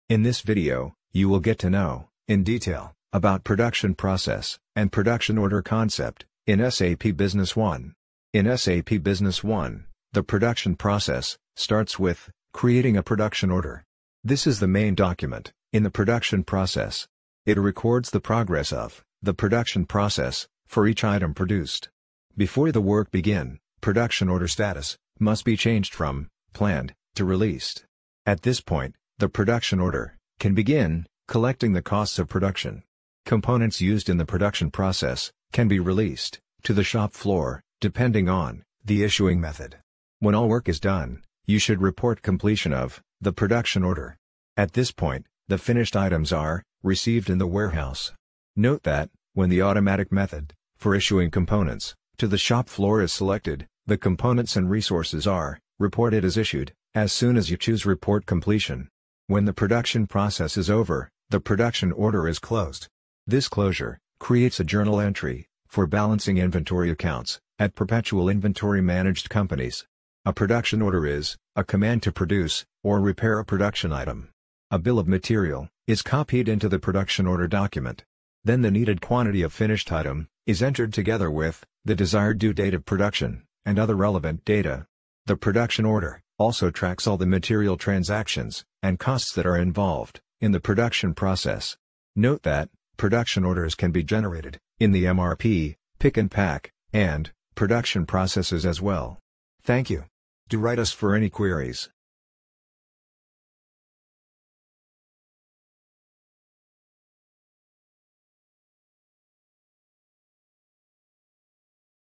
Production Process And Production Order Concept In Sap Business One | Emerging Alliance
In SAP Business One, The production process starts with creating a Production Order. This is the main document in the production process. It records the progress of the production process for each item produced.
Before the work begin, Production Order status must be changed from “planned” to “Released”. At this point, the Production Order can begin collecting the costs of production.
Components used in the production process can be released to the shop floor (depending on the issuing method).
When all work is done, you should report completion of the production order. At this point, the finished items are received in the warehouse.
Note that when the automatic method for issuing components to the shop floor is selected, the components and resources are reported as issued as soon as you choose report completion.
When the production process is over, the Production Order is closed. This closure creates a journal entry for balancing inventory accounts at perpetual inventory-managed companies.

A Production Order is a command to produce (or repair) a production item.
A Bill of Material (BOM) is copied into the Production Order document. Then the needed quantity of finished item is entered together with the desired due date of production and other relevant data.
The Production Order also tracks all the material transactions and costs that are involved in the production process.
Note that production orders can be generated in the MRP, Pick and Pack and Production processes as well.